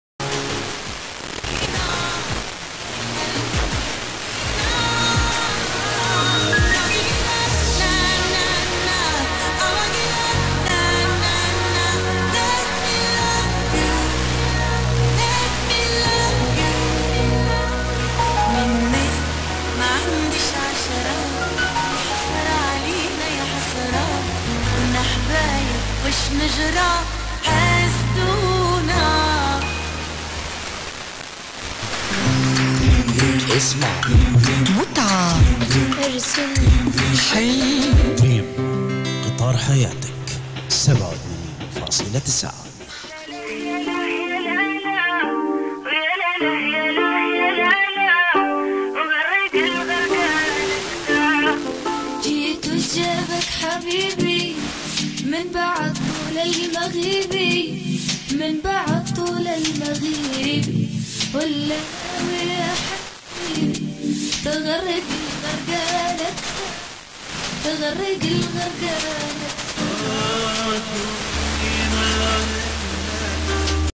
[Sporadic-E] Dream FM Libya on 87.9 MHz
Full ID at 00:30
TX: Tripoli, Libya
QTH: Székesfehérvár, Hungary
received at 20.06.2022